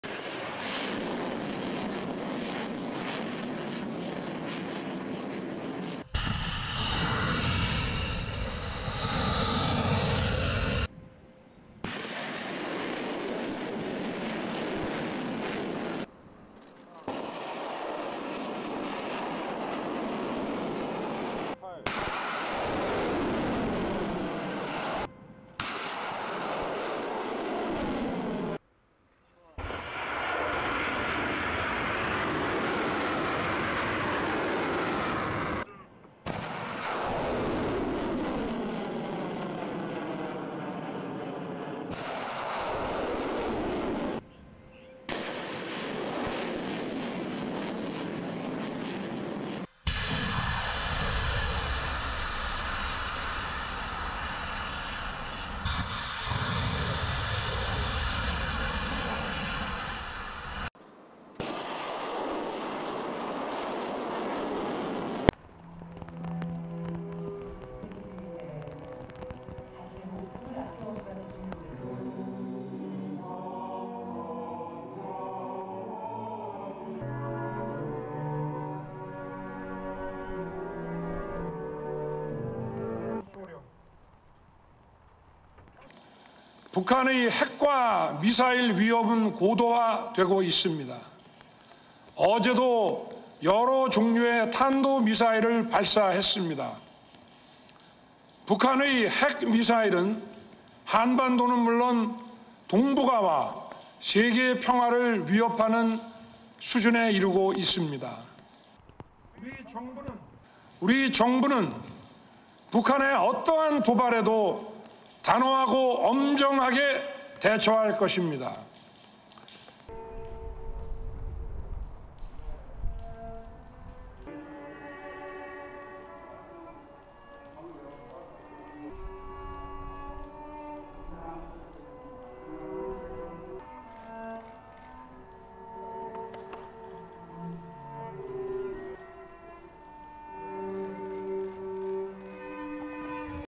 S. Korea and US fire eight missiles in tit-for-tat move to warn the North's Kim
South Korean President Yoon Suk Yeol on Monday said his government would pursue "fundamental and practical security capabilities" to counter North Korea's growing nuclear weapons and missile threat.

"North Korea's nuclear weapons and missile programs have grown to a point where they are not only a threat to the Korean Peninsula, but to Northeast Asia and world peace," Yoon said at the National Cemetery in Seoul, during a ceremony to mark the Memorial Day holiday.

He added his government would "sternly respond to any kind of North Korean provocation."